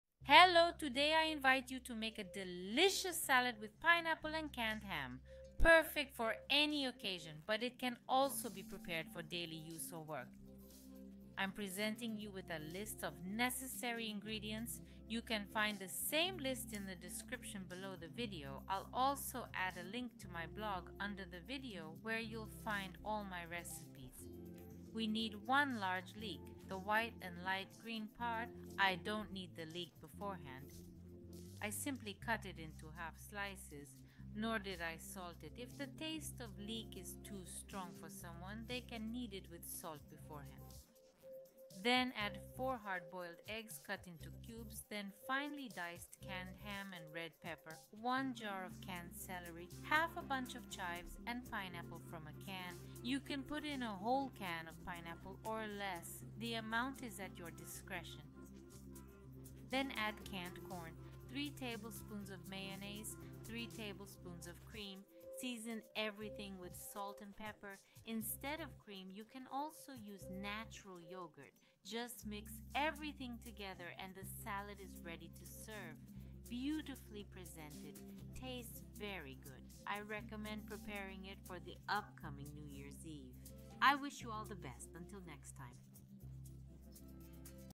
Salad with ham and pineapple - delicious and colorful
Ingredients:
Large leek (white and light green part)
4 eggs
Canned pineapple
Canned corn
Canned ham 250 g
1/2 red pepper
Small bunch of chives
Pickled celery
3 tablespoons of mayonnaise
3 tablespoons of cream or natural yogurt
Salt
Pepper